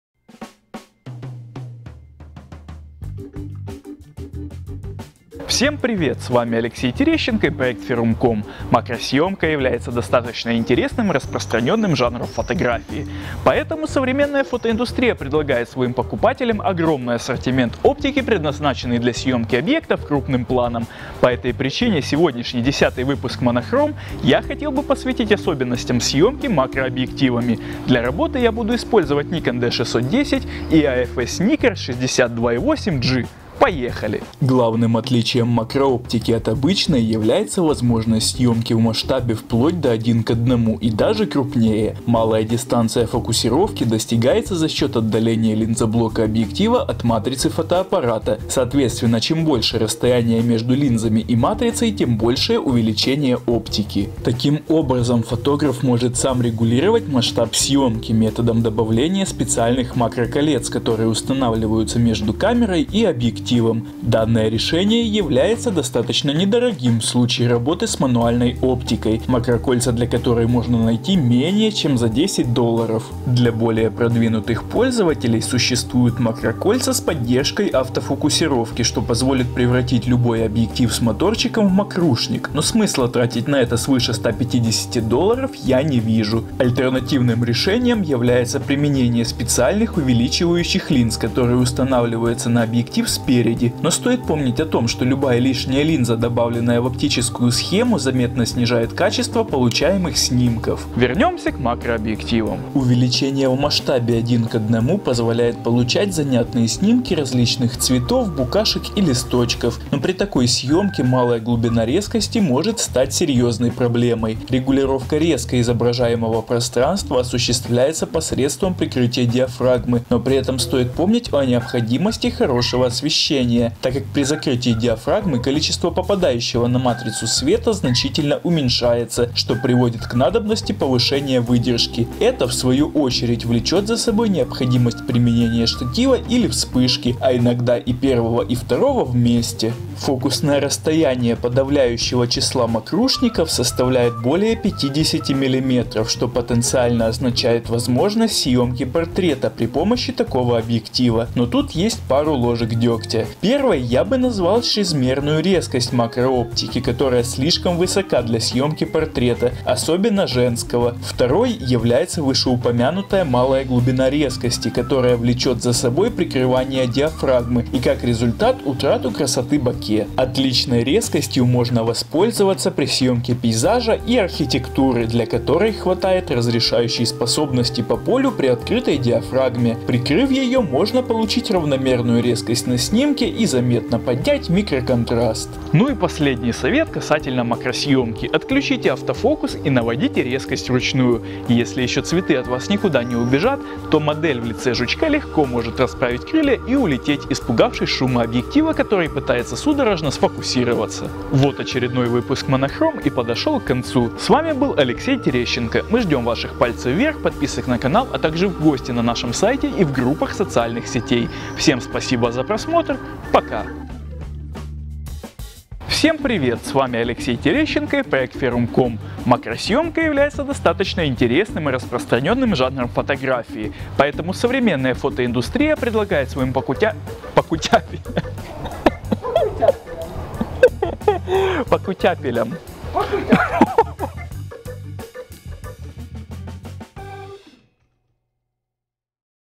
Работа с макрооптикой: monochrome #10 от FERUMM.COM
monochrome #10! Приветствуем всех любителей поснимать различных жучков. В этом выпуске мы поговорим о приёмах съёмки объективами, специализированных для макрофотографии, а также узнаем об альтернативных методах съёмки крупным планом.